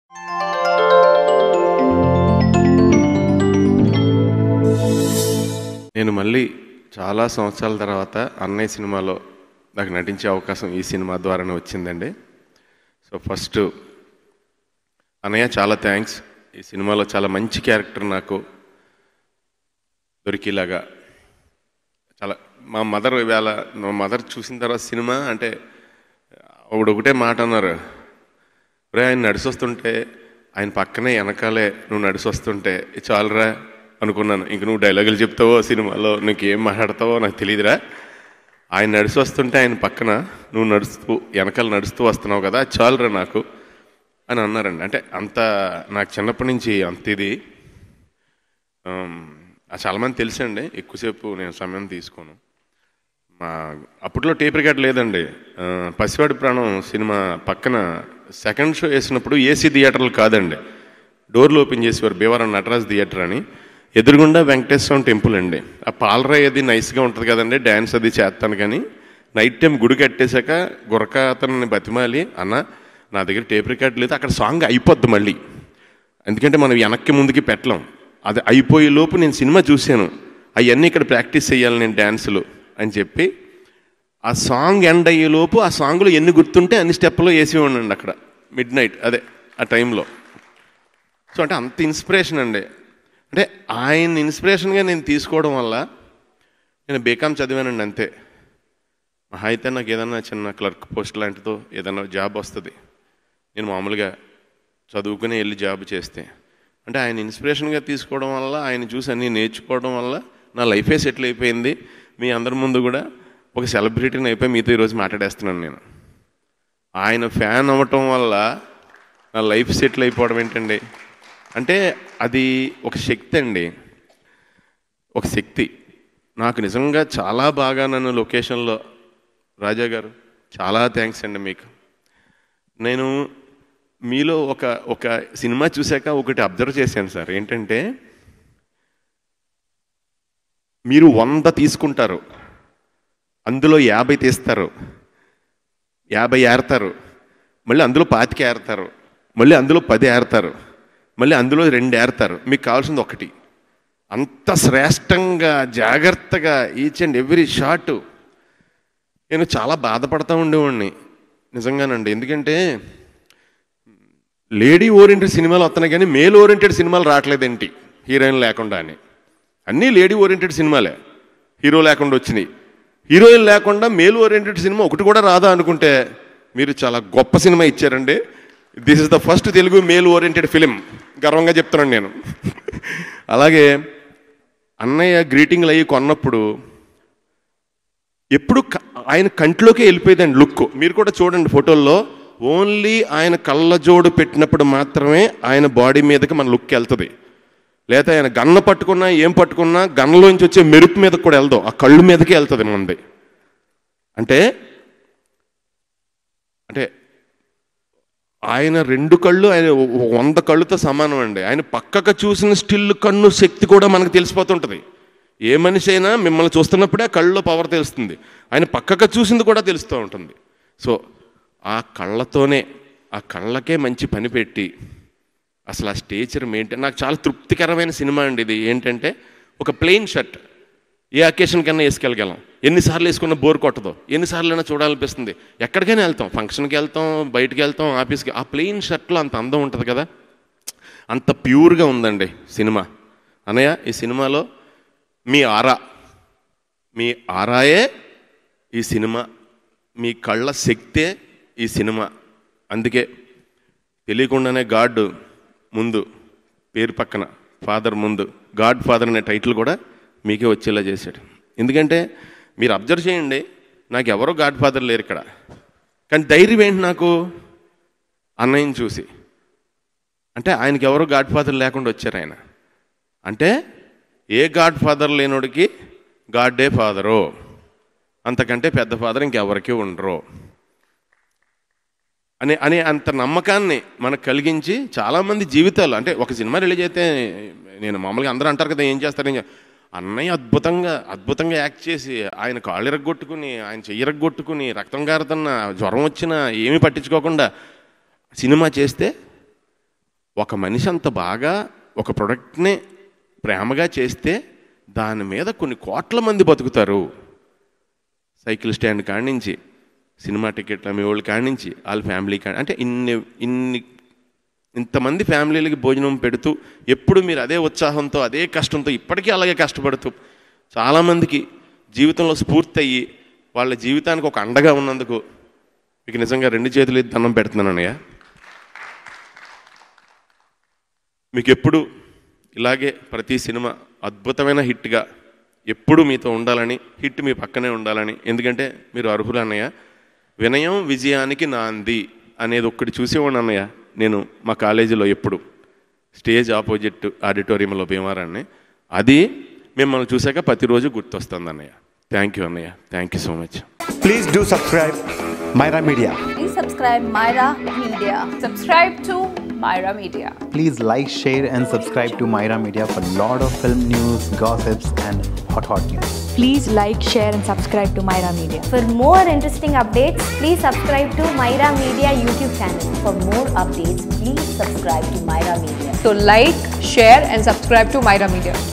ఎప్పుడూ హిట్ మీ పక్కనే ఉండాలి Actor Sunil Superb Speech @ Godfather Blockbuster Success Meet
Some of our top Playlists you might be interested